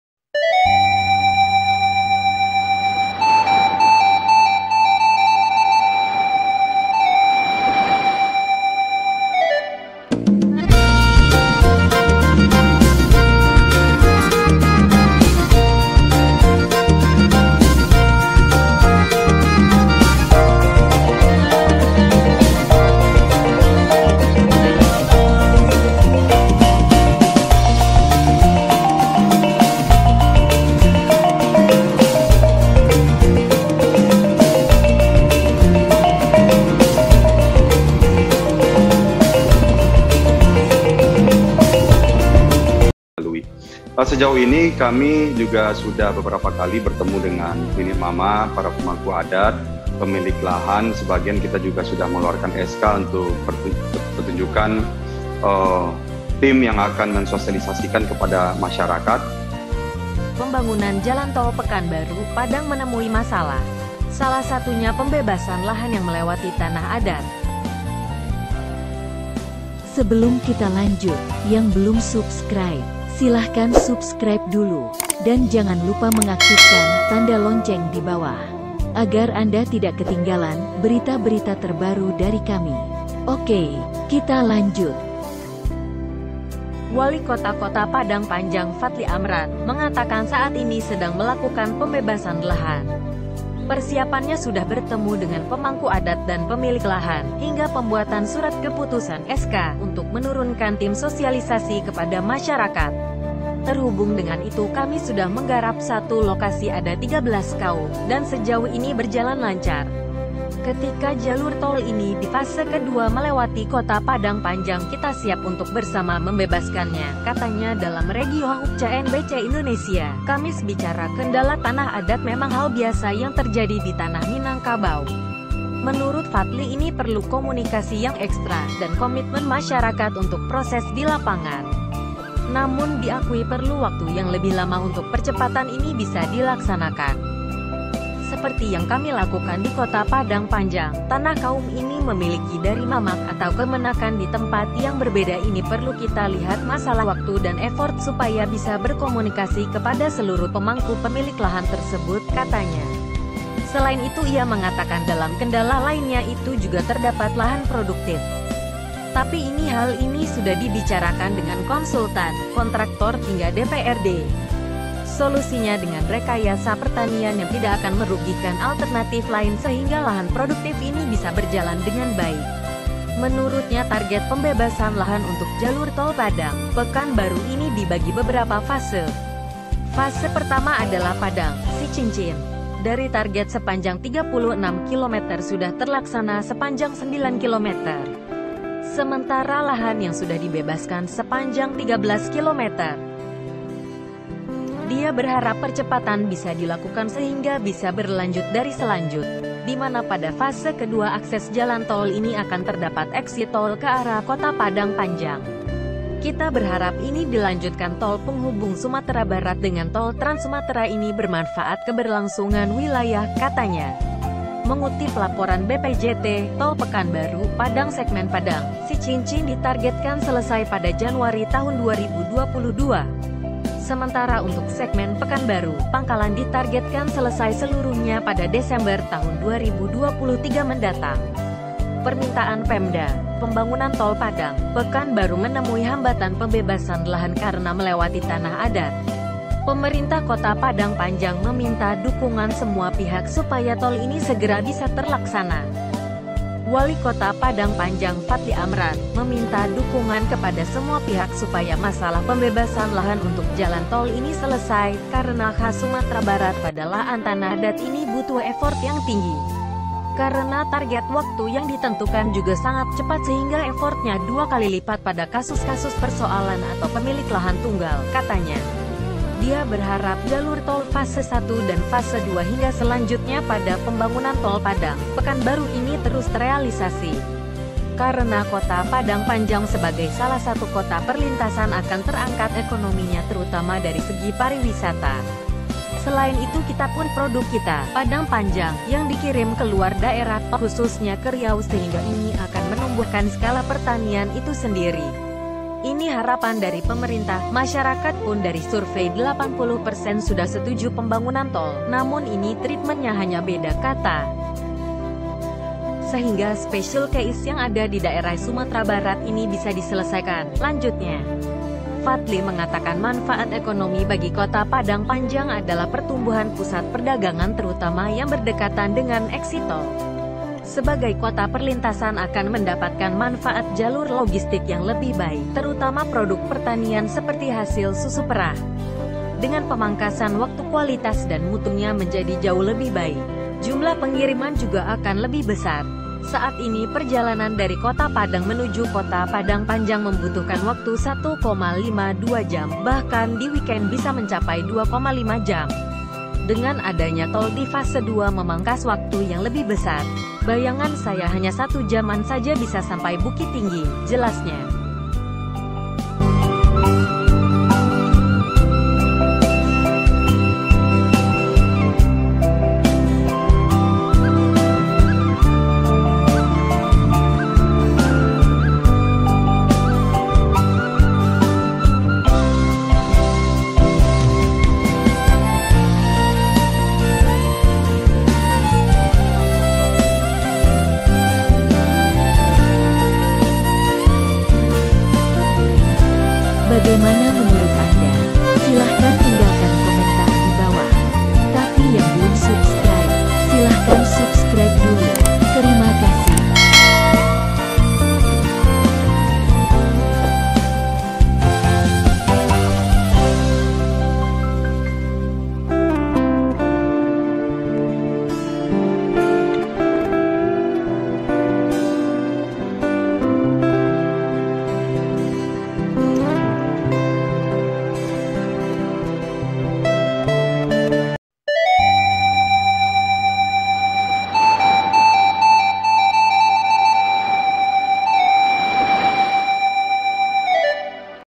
Update Proyek Tol Pekanbaru Padang, Walikota Padang Panjang Siap Pembebasan Tanah
Walikota Padang Panjang Fadly Amran, meminta dukungan kepada semua pihak supaya masalah pembebasan lahan untuk jalan tol, karena khas Sumatera Barat pada lahan tanah adat ini butuh effort yang tinggi.